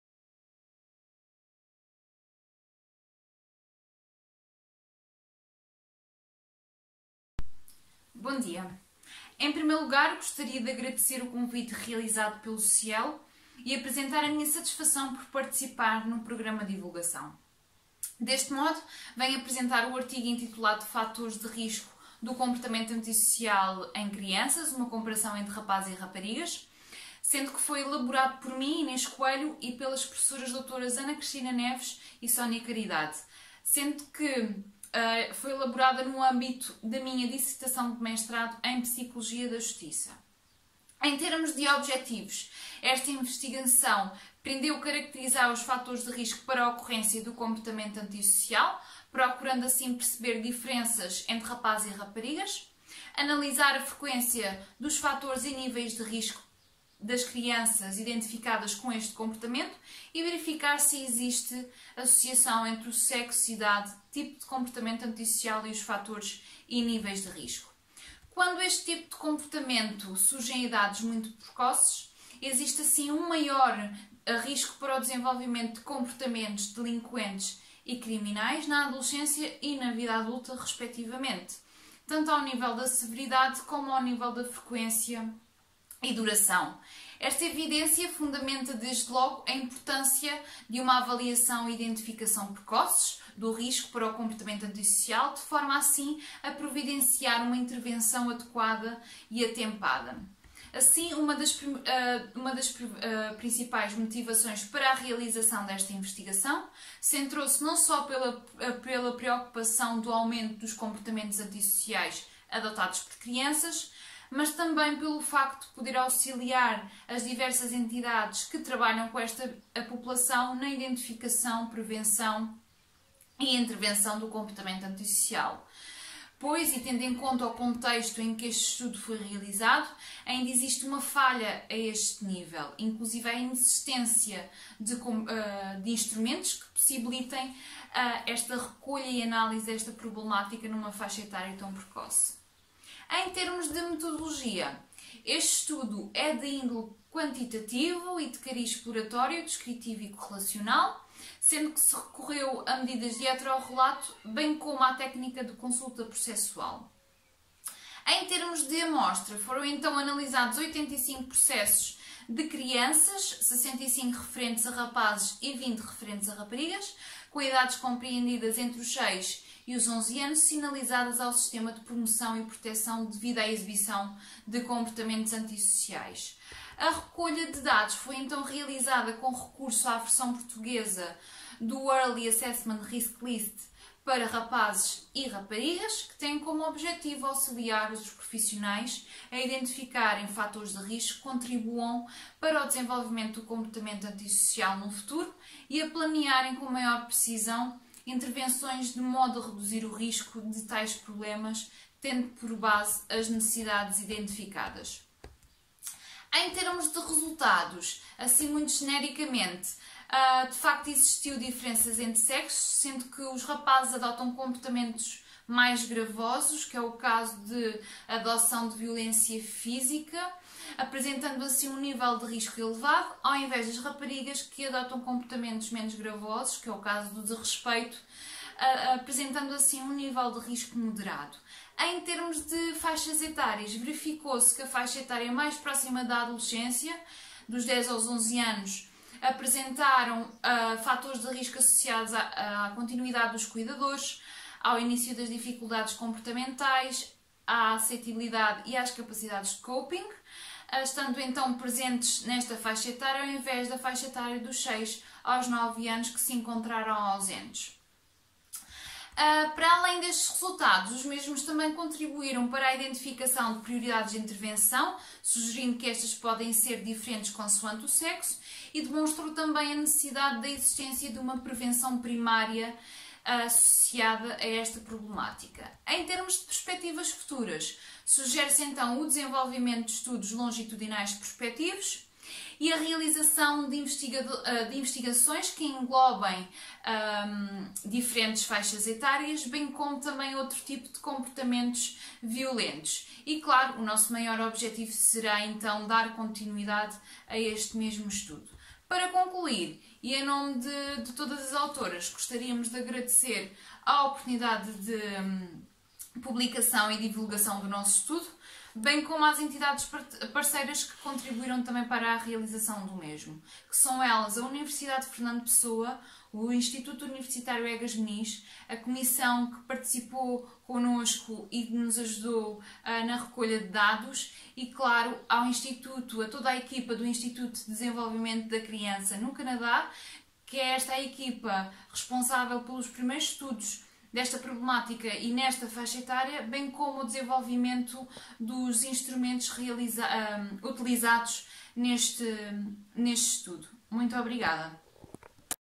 Comportamento antissocial tem um papel impactante na vida de crianças dos 6 aos 11 anos
Estudo revela a importância da prevenção primária e da identificação de prioridades de intervenção, focadas, essencialmente, nas diferenças entre sexos, contextos e populações em crianças em idade escolar, dos seis aos onze anos, com comportamentos antissociais. Neste sentido, a prevenção e intervenção realizadas num período precoce na vida das crianças, permitirá a redução dos comportamentos delinquentes e criminais no futuro.